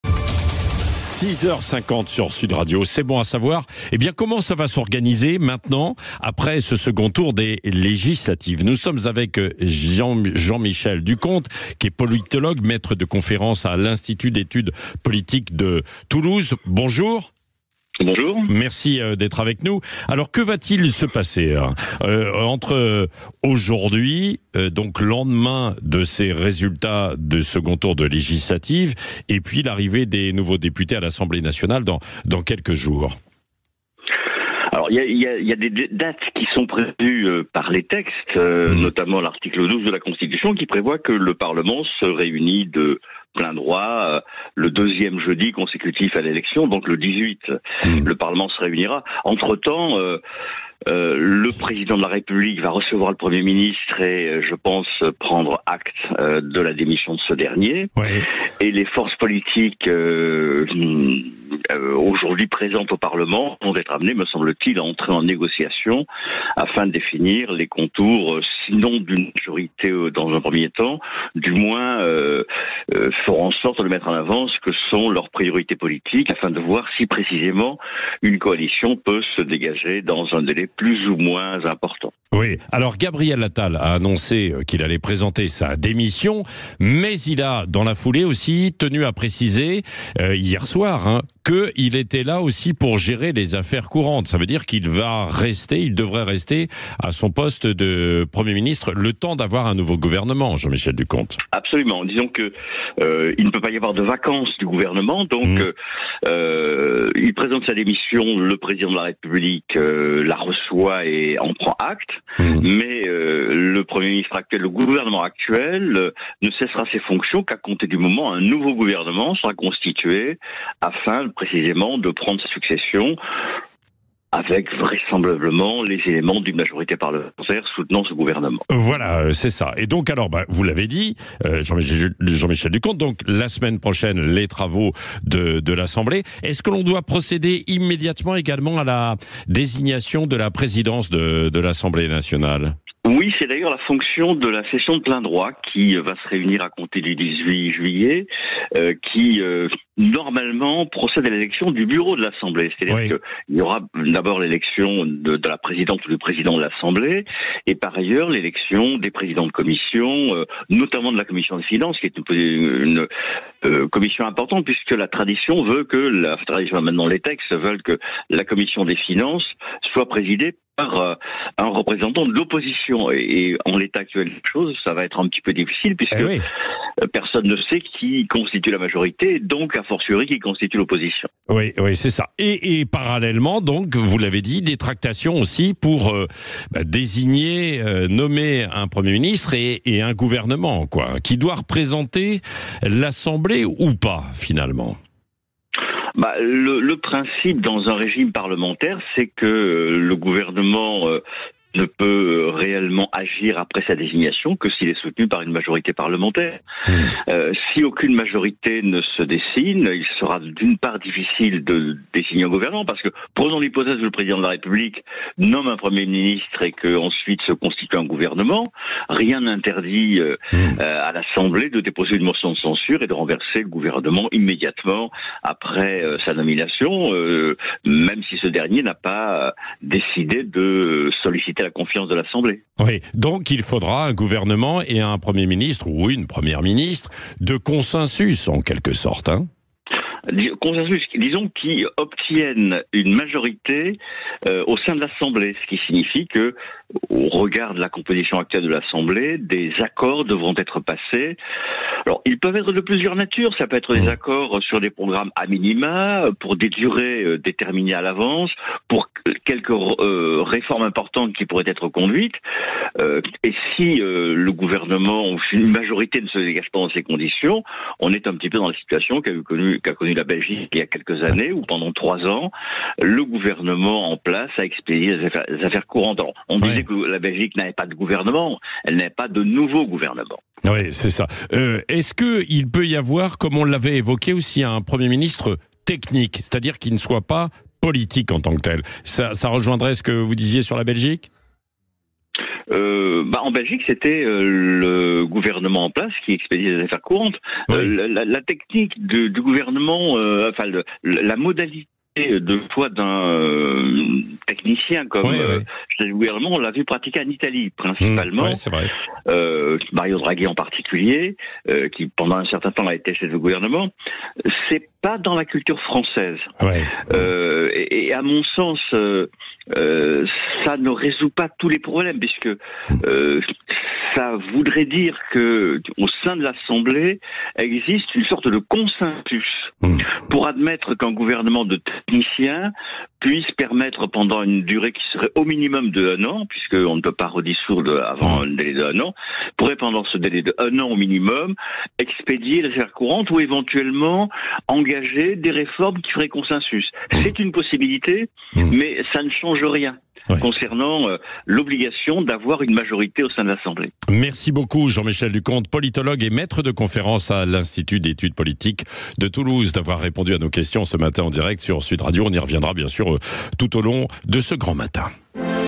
Que va-t-il se passer après le second tour des élections législatives ?
Avec Jean-Michel Ducomte, politologue et Maître de conférences à l’Institut d’Etudes Politiques de Toulouse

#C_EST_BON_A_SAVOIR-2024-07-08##